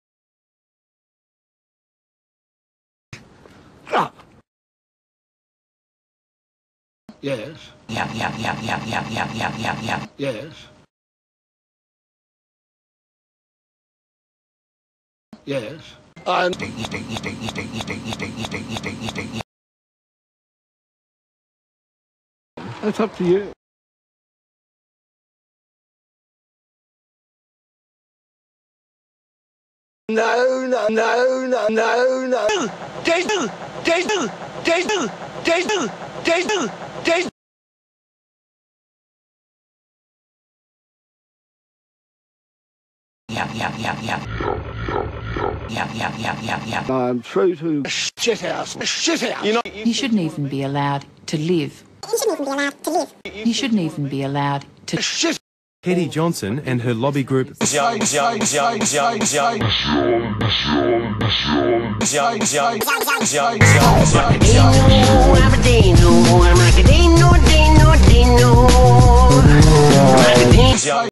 Geoff on Food
Geoff talks about food but then goes a bit loopy. Featuring Petty Johnson.